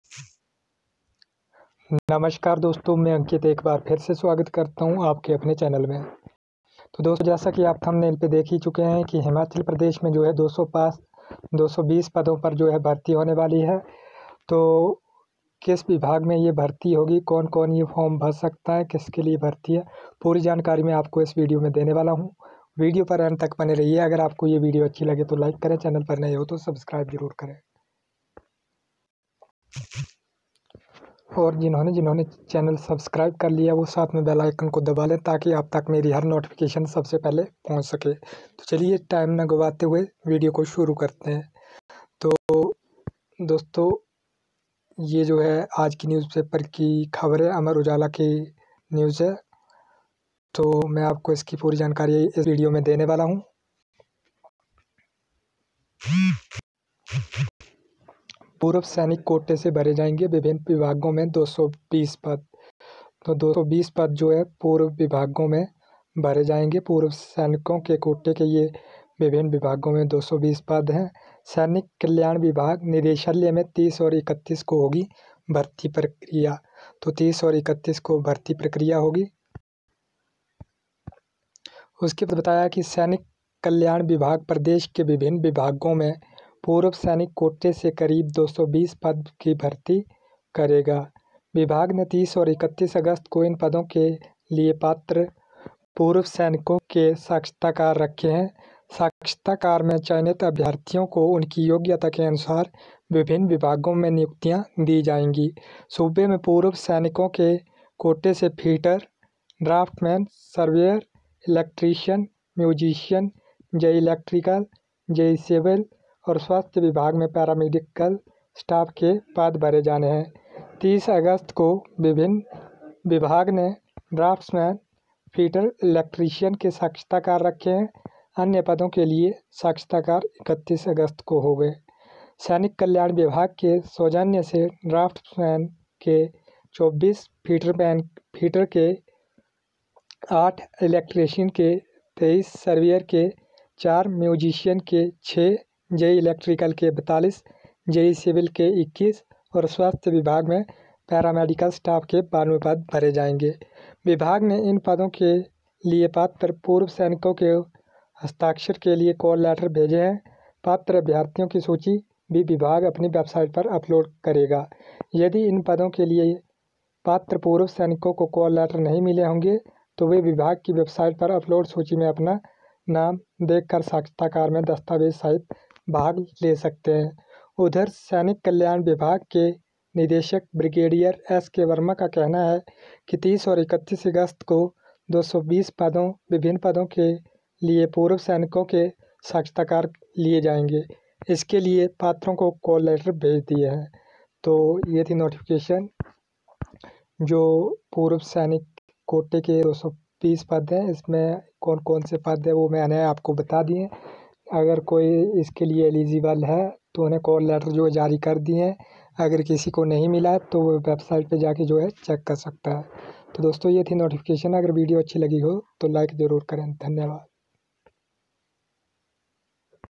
हिमाचल प्रदेश में होगी 220 पदों पर भर्ती कोन भर सकता है ये फॉर्म कोन कोन पद भरेंगे जाने पूरी जानकारी
हिमाचल प्रदेश में होगी 220 पदों पर भर्ती कोन भर सकता है ये फॉर्म कोन कोन फपद भरेंगे जाने पूरी जानकारी